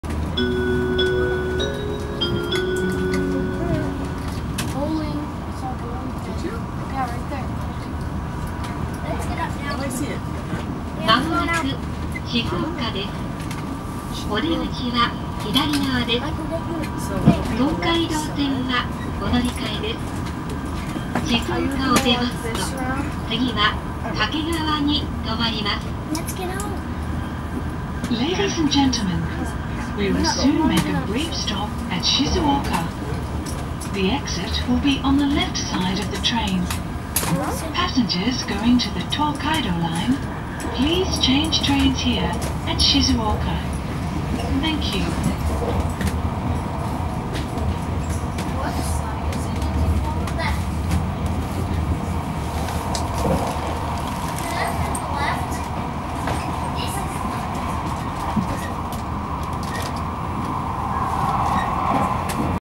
新幹線から静岡ガンダム
こだまE席から撮影